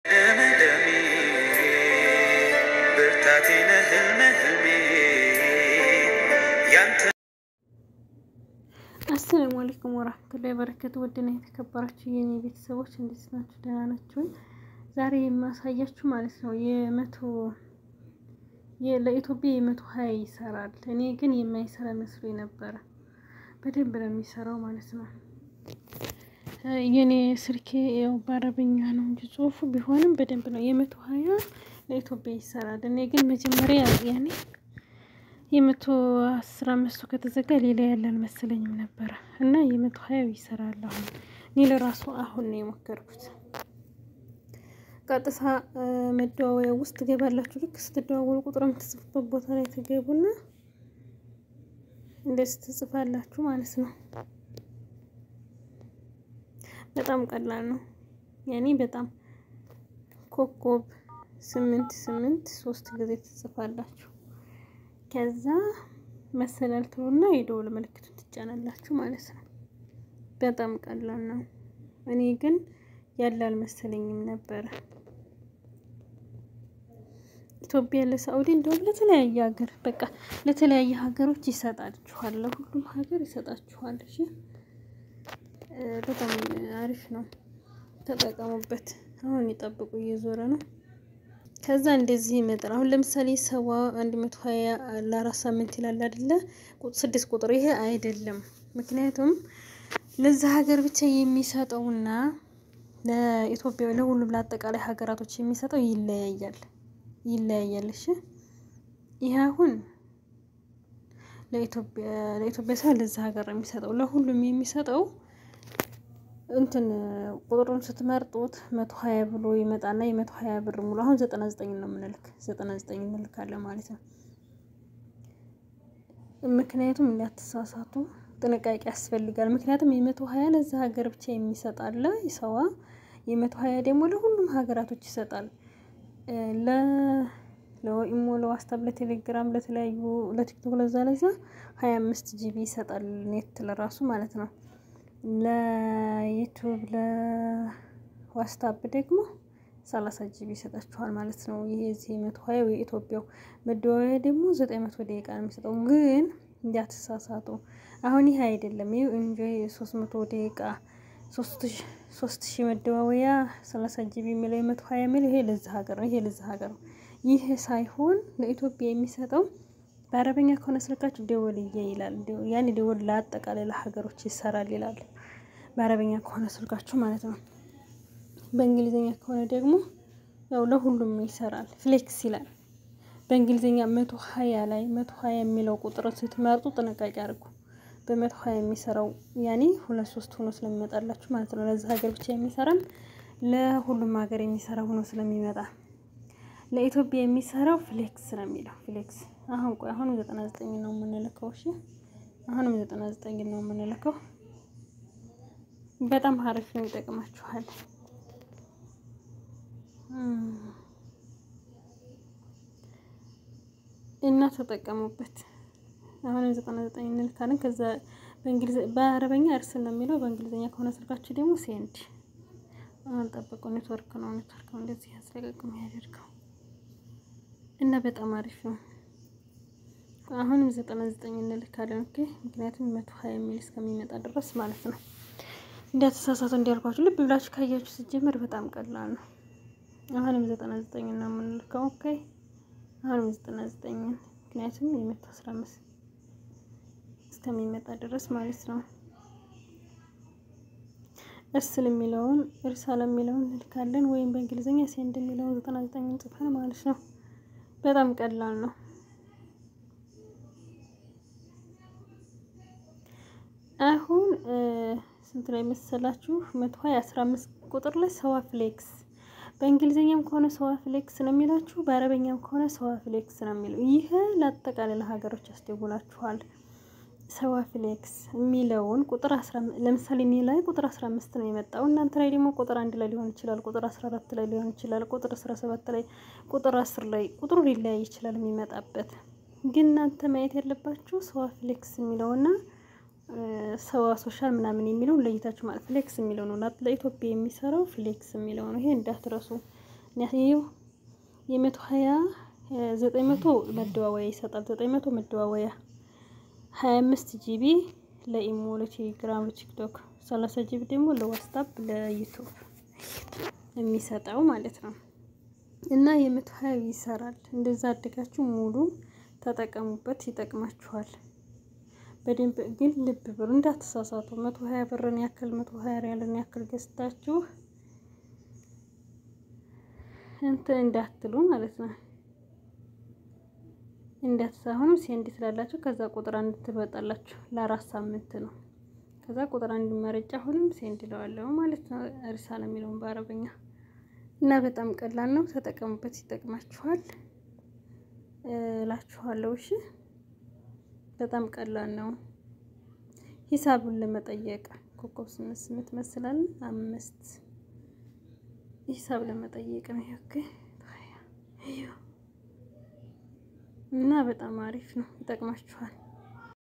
የ120ሪያል ሰዋ ካርድ ለኢትዮጲያ መጃን ይሰራል እኮን ደስስስስስ አላችሁ መብሩክ
ሰዋ ካርድ የመቶ ሀያ እሪያል ይሰራል ለኢትዮጲያ